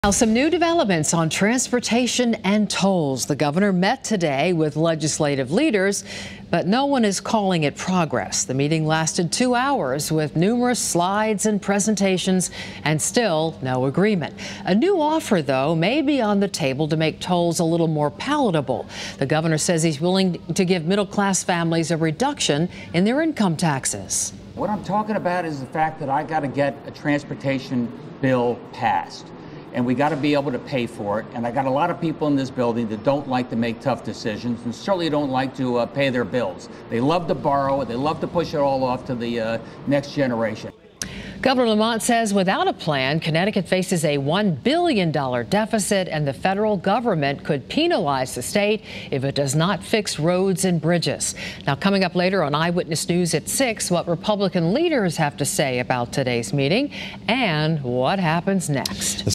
Video: Governor, lawmakers meet to discuss transportation, tolls
The governor met with a bipartisan group of legislative leaders to discuss transportation and tolls on Wednesday.